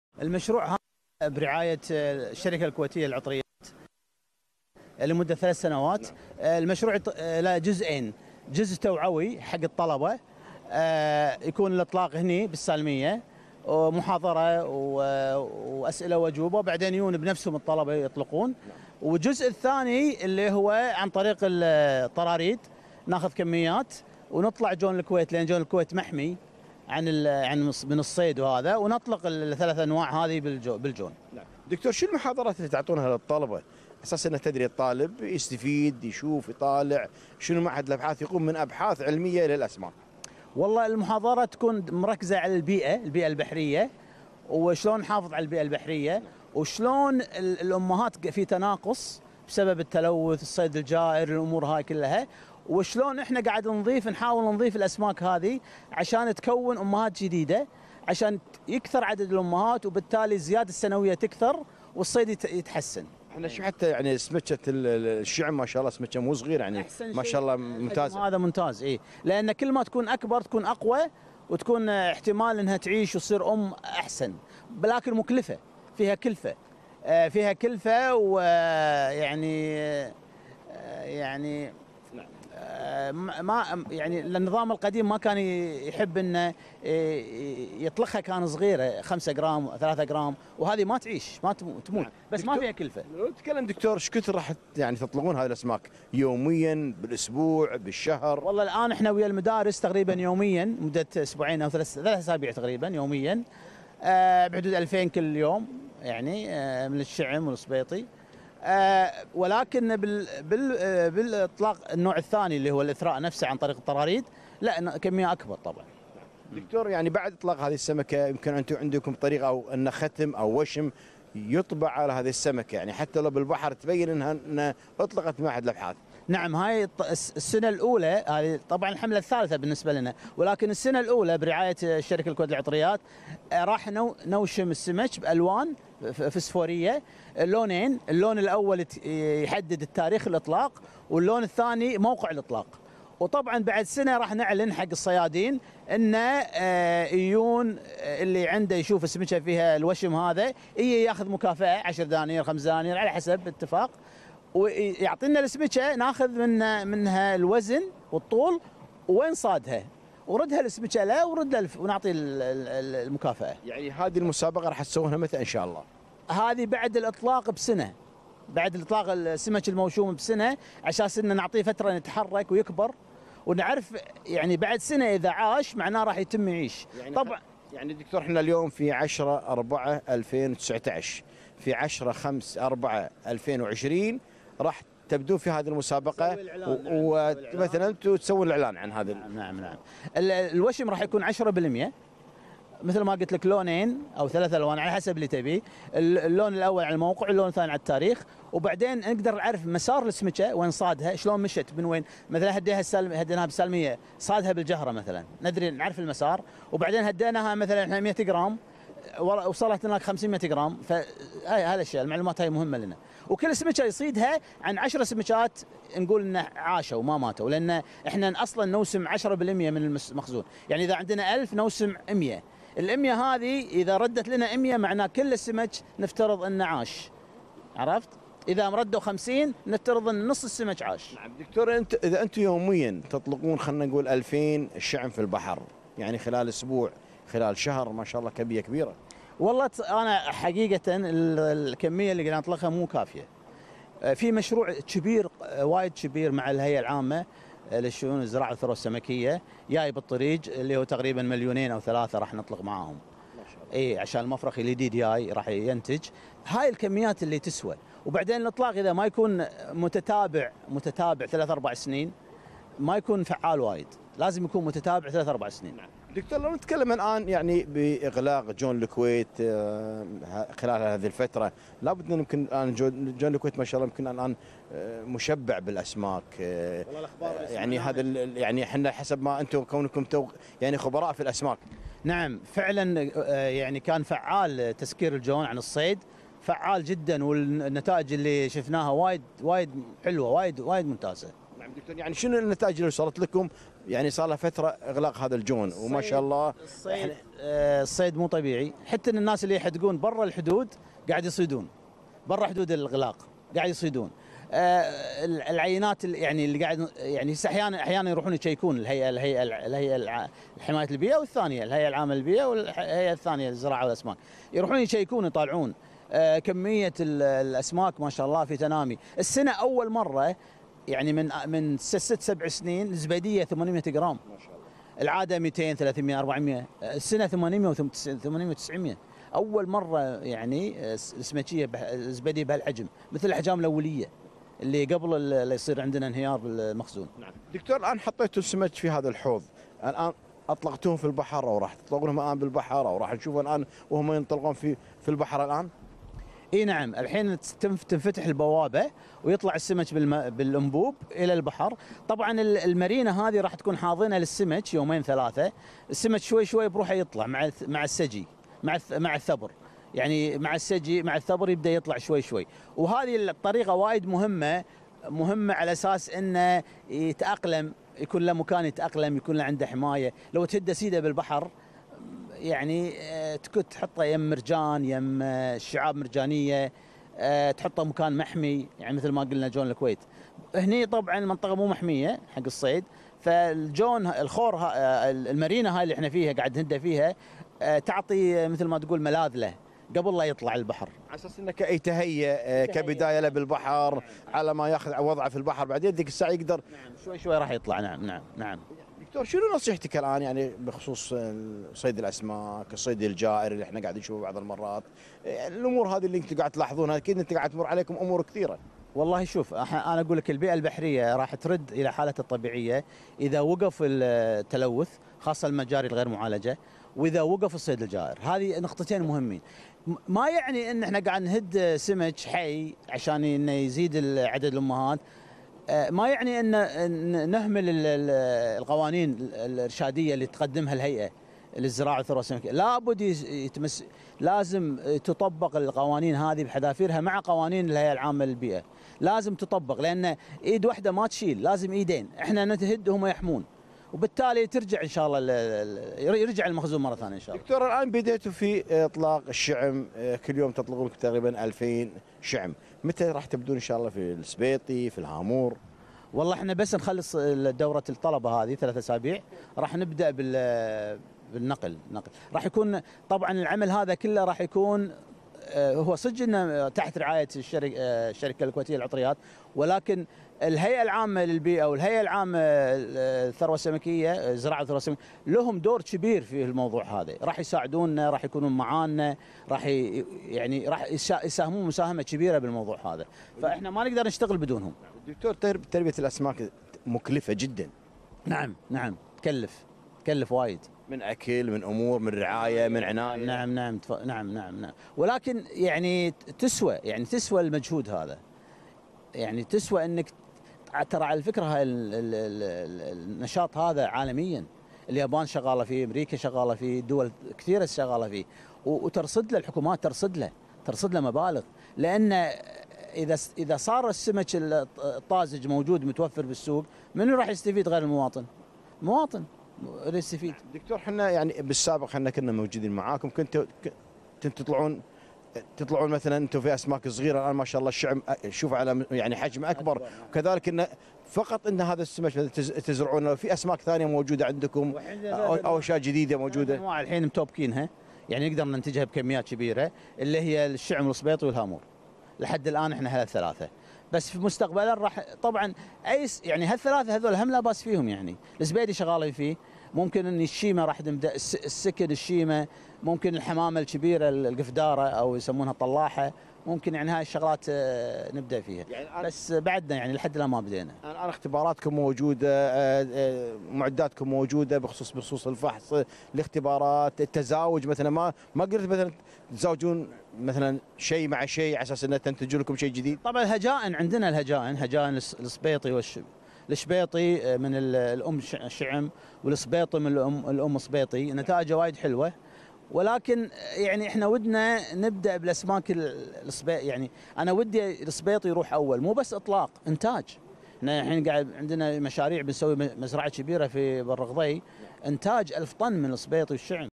لقاء مع د. خالد العبد الإله من مركز أبحاث البيئة والعلوم الحياتية
الحديث حول حملة إثراء المخزون السمكي الثالثة